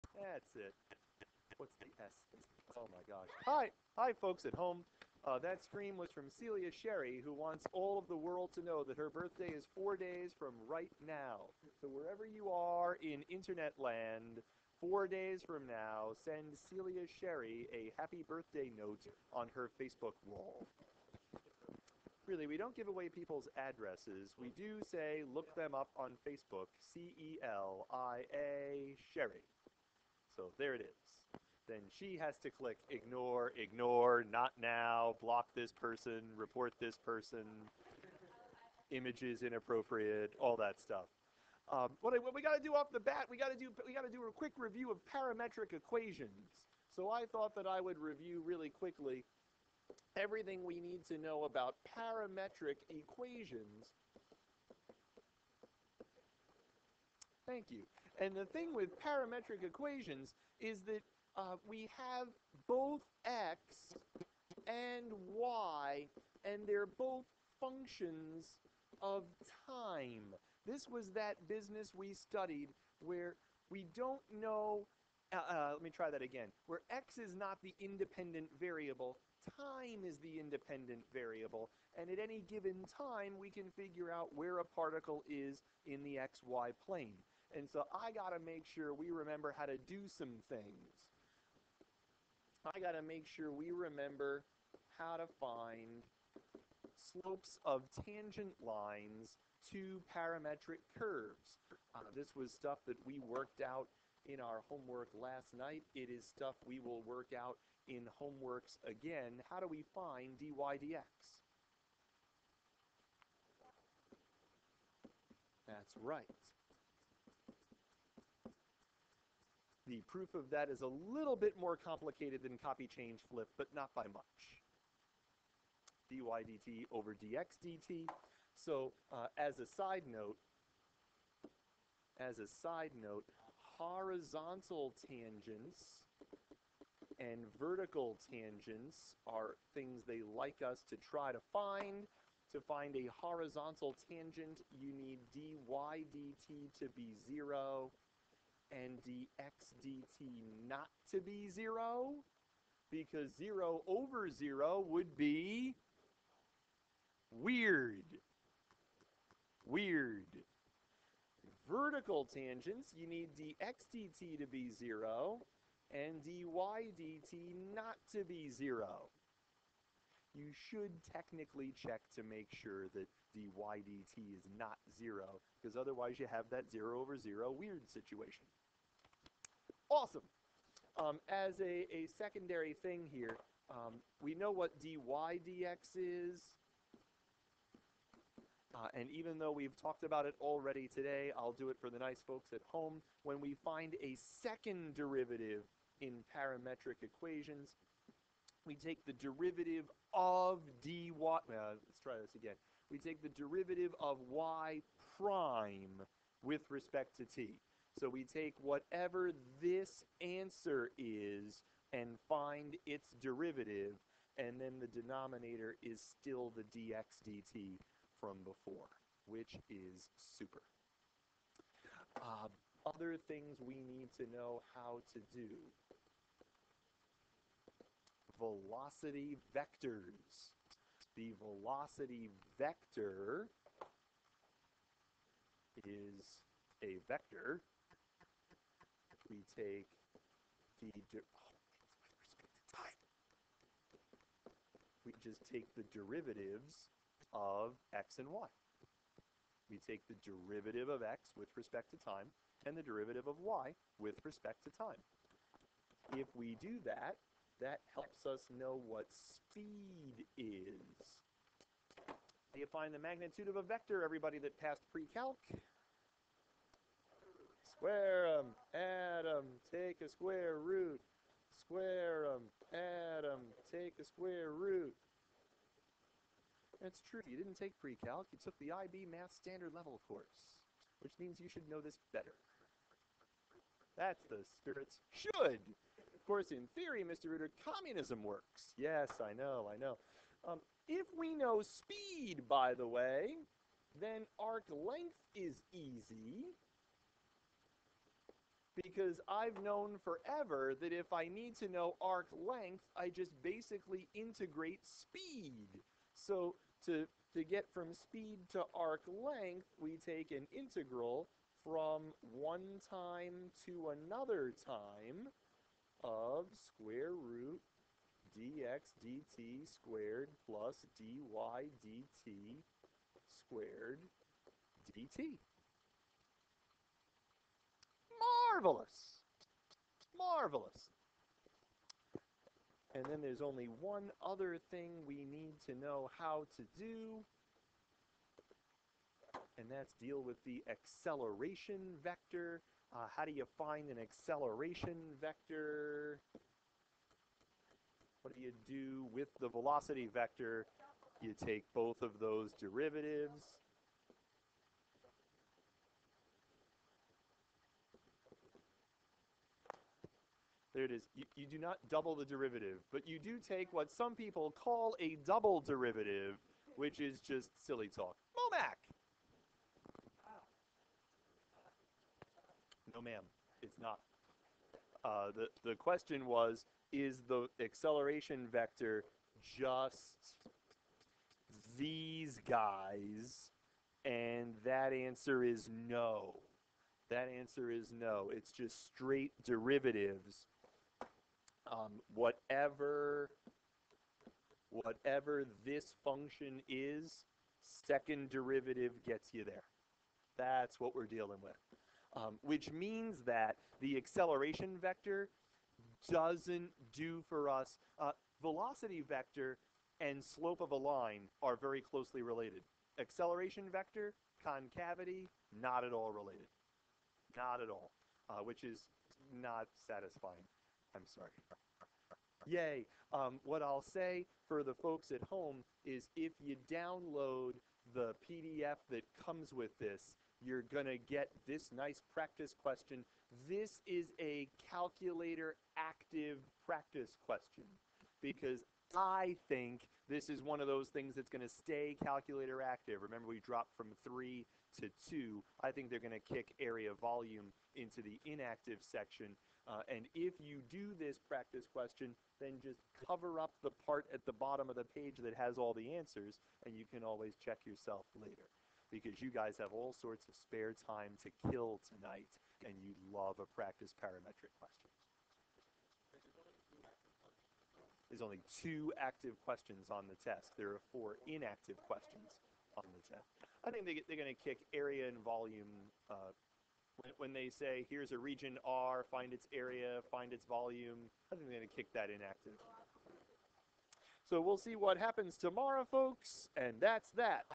CA Calculus BC Refresher - Parametric Equations
This is a quick review of some things you'll need to know regarding parametric equations if you're taking the AP Calculus BC exam in May.